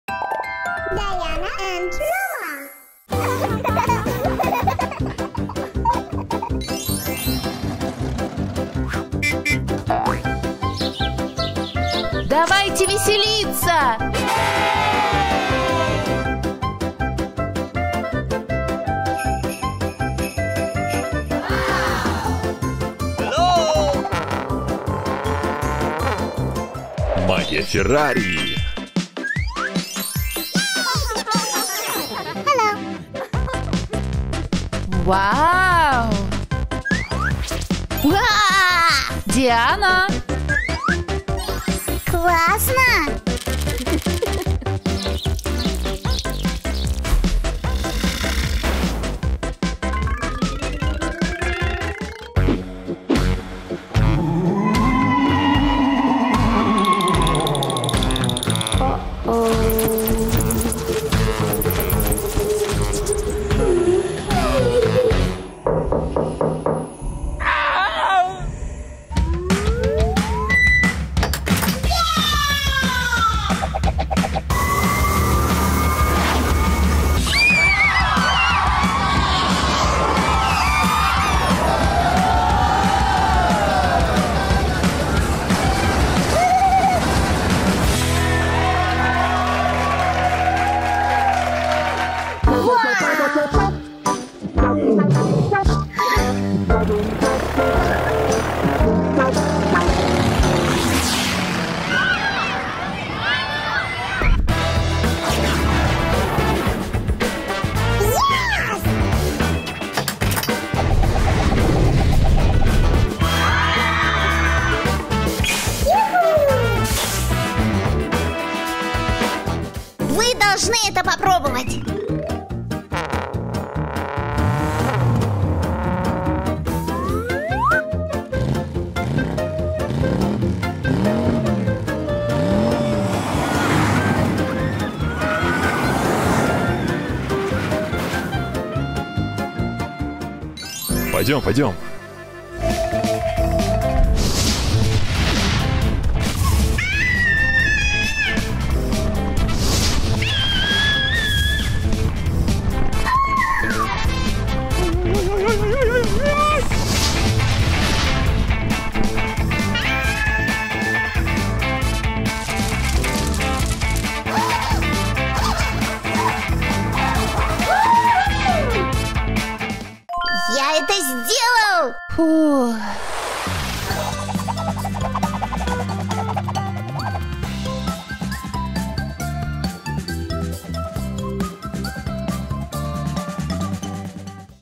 Диана и Рома Выходные в парке Ferrari World в Абу-Даби
В выходные Диана и Рома вместе с родителями посетили тематический парк Ferrari World в Абу-Даби. Дети провели целый день со своими родителями, катаясь на аттракционах. Это был незабываемый день приключений!
Kids Diana Show for kids